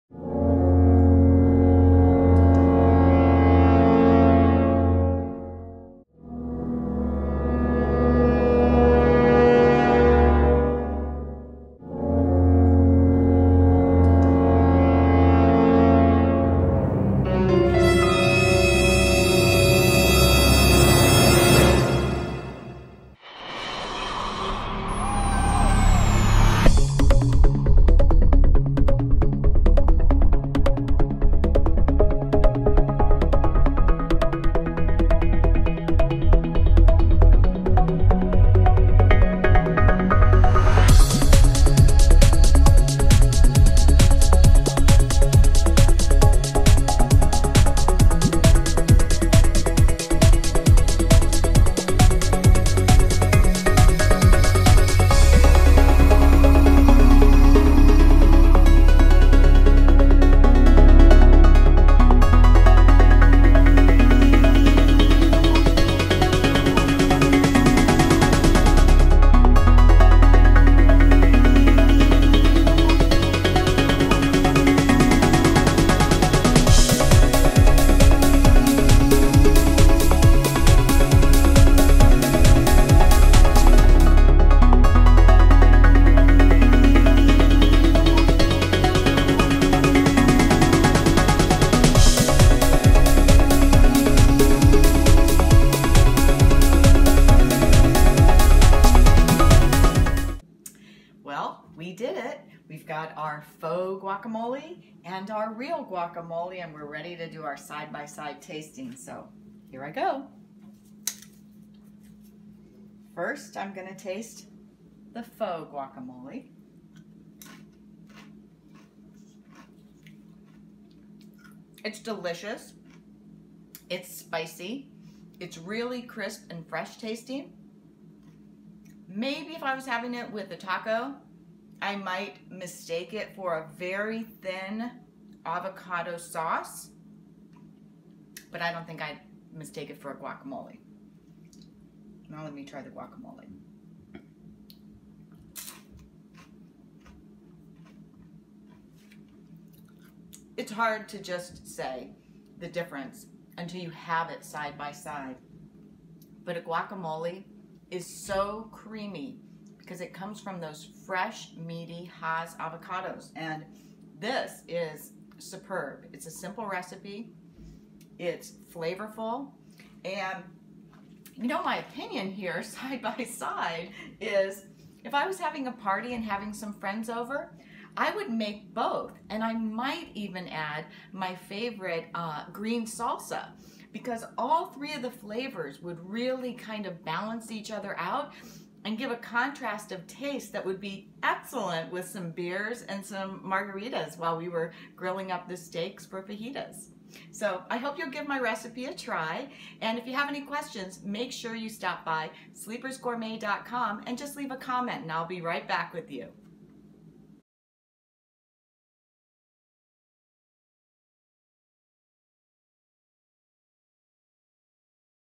Guac or Not? Faux-amole Recipe
A dip that's sure to impress and surprise your guests! Create this delicious faux-amole recipe with Christina!